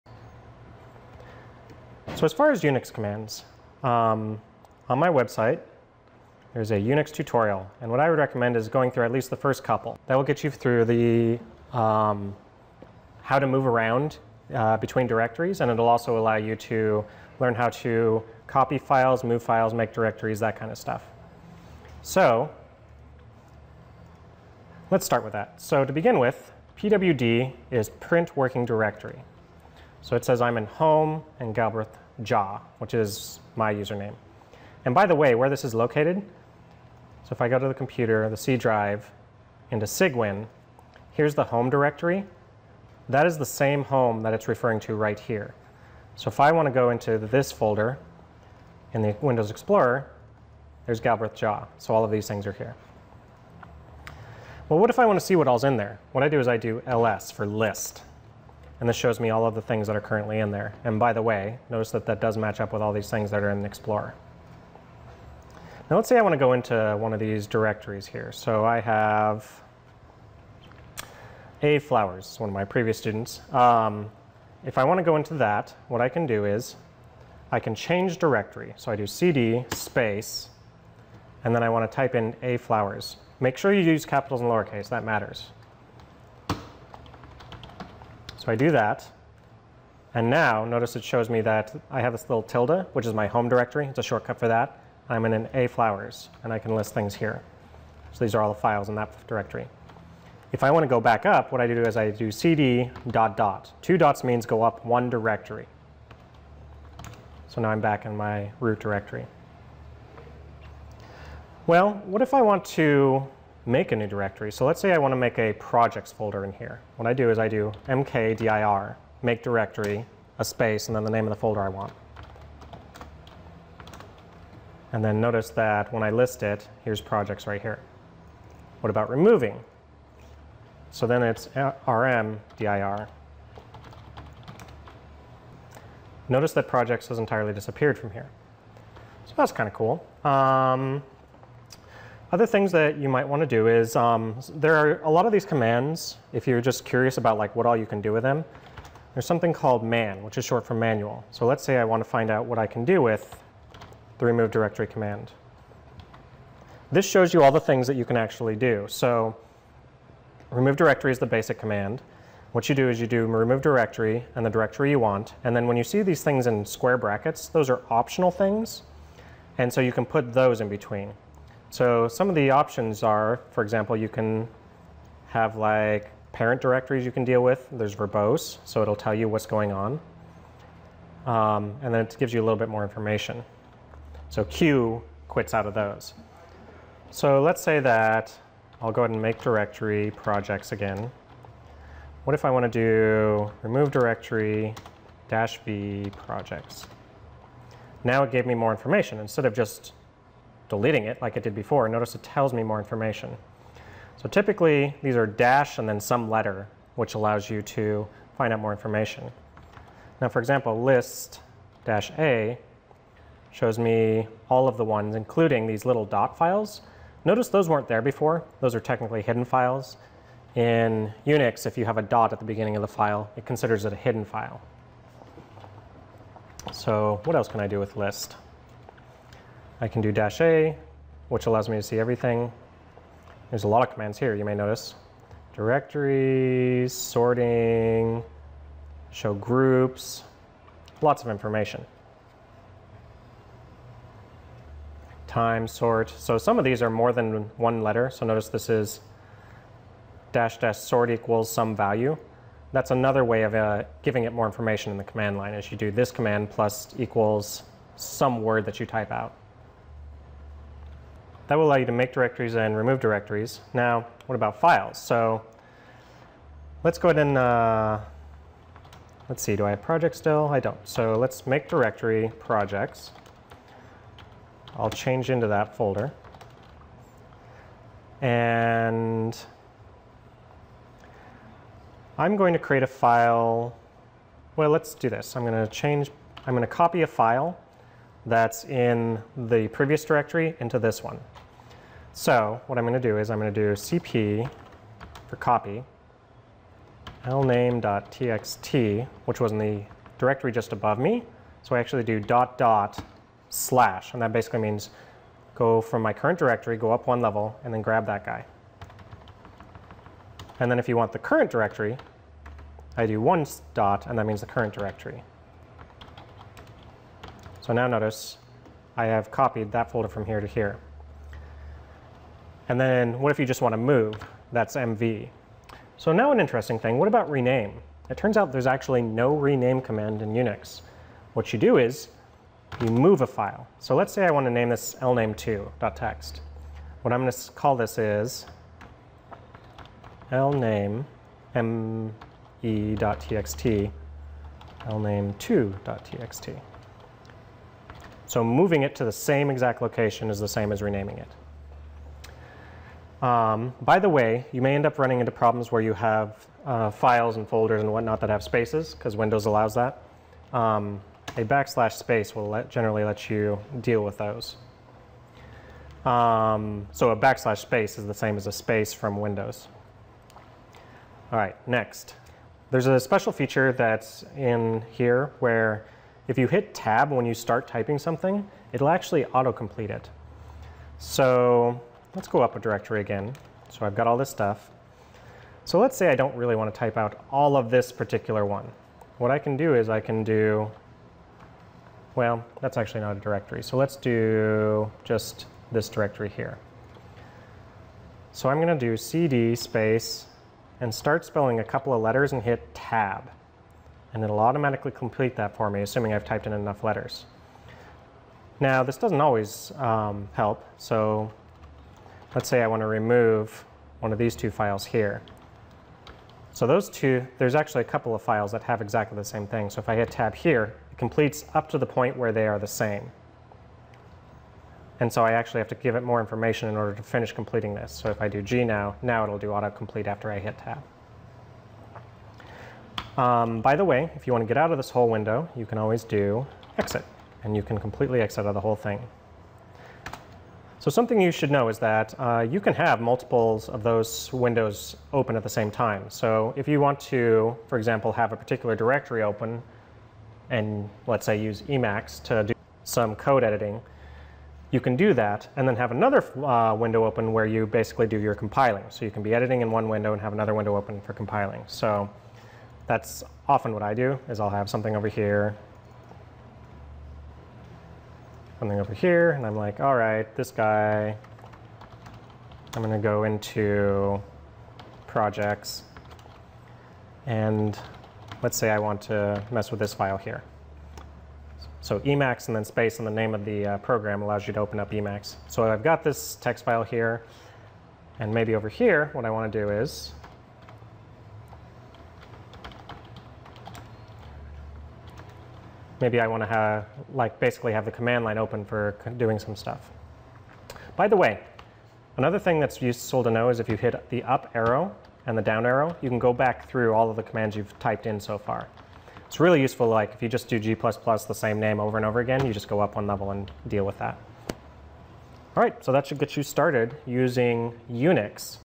Unix
Basic Unix commands to move around the file directory, make directories, and remove directories.